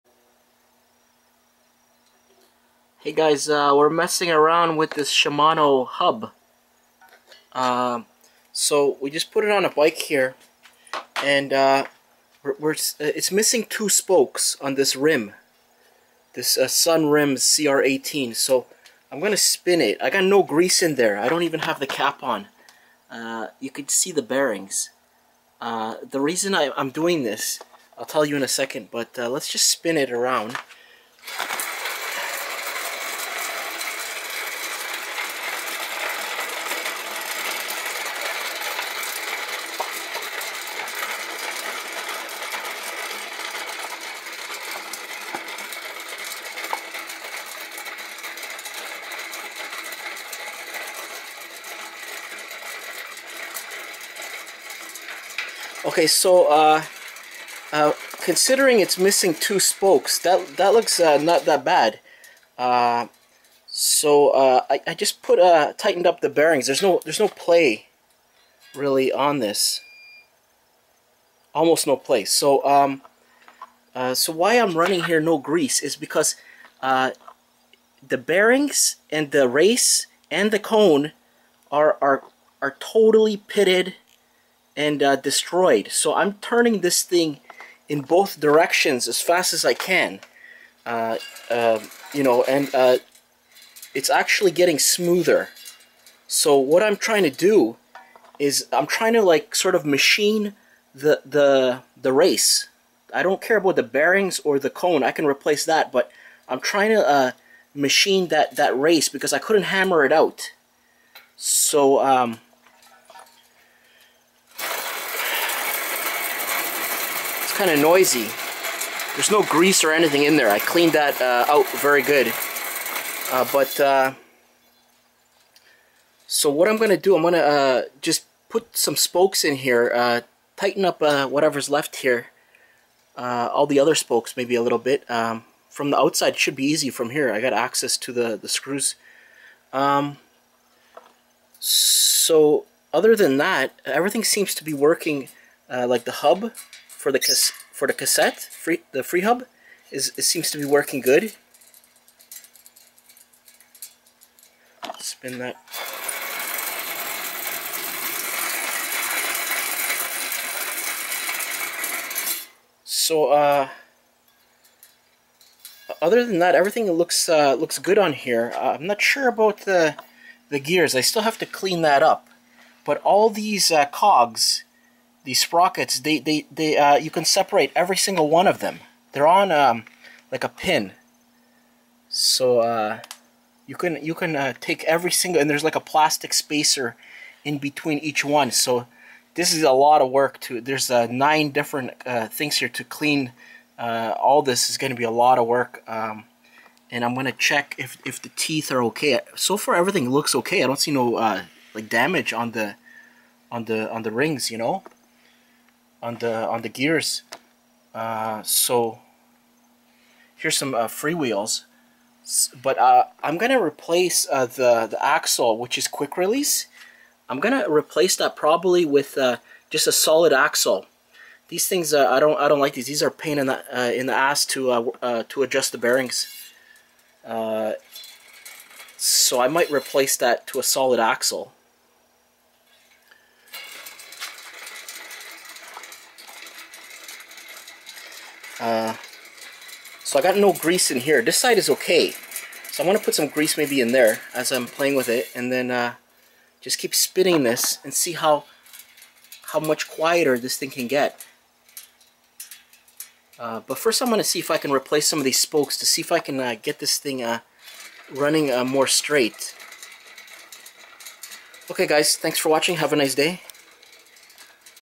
Service shimano hub and wheel Part 1 FH-M475 VIAM
Part 1 Service shimano hub and wheel. wheel is not true, 2 missing spokes, non drive side bearings are pitted, cone trashed, hub race trashed. can we fix it ! ?  FH-M475 VIAM hub with sunrims cr-18 rim 26" x 19 mm inner width